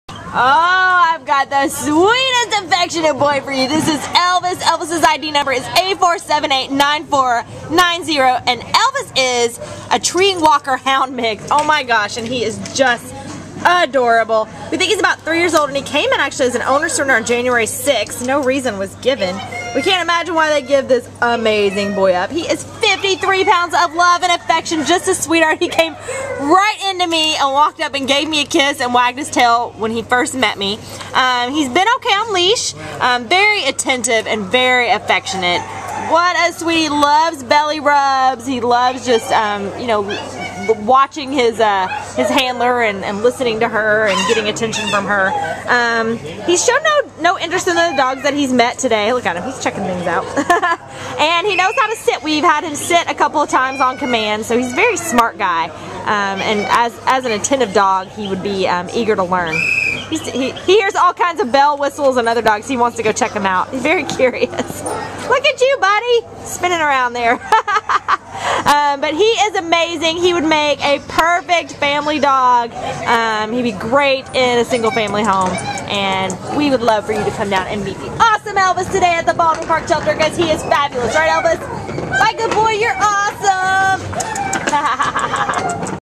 A4789490 Elvis
A4789490 Elvis is a sporty 3-year-old white-and-red male Treeing Walker Coonhound who was surrendered to the Baldwin Park Animal Care Center on January 6th because, his owner said, he does not get along with other pets. Weighing 53 lbs, Elvis (like his namesake) is truly one-of-a-kind! A real sweetheart who immediately gave his handler a kiss when they first met, his tail wags most when he is swapping affection with people. He is attentive too. In fact, he already walks capably on leash and even knows how to sit on command! As much as he loves people, though, he has no interest in other dogs and needs to be the only pet in his household. He is sure to be enough fun for the whole family, though, since Elvis’s athletic energy will make him the perfect activity buddy!